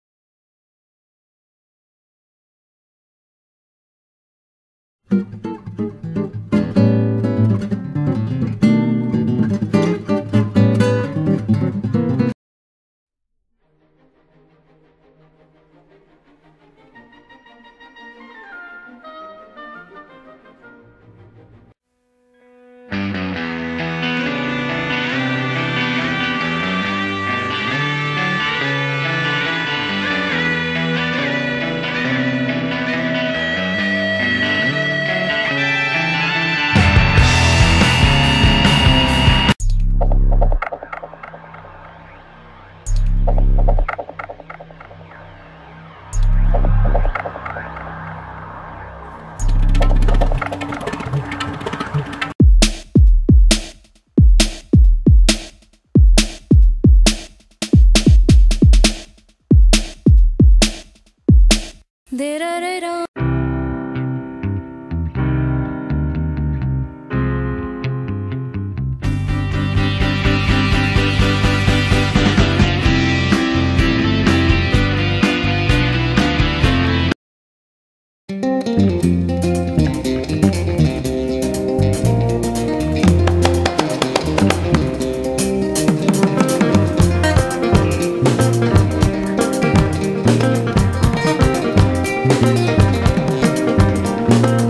Anime show I want to make.
Tjhis is a anime show I want to make and I know there are charters in here from other shows. I would not use them in the show its just I can't really drawl so I am saying I would want them to look close to this. If you have any questions about anything please ask. Oh and the reason I have old music is one because it fits the story time line and also I had to erase all history in my computer to fix it and now it won't let me upload my music into my computer. So just bare with me on this until I figure this out.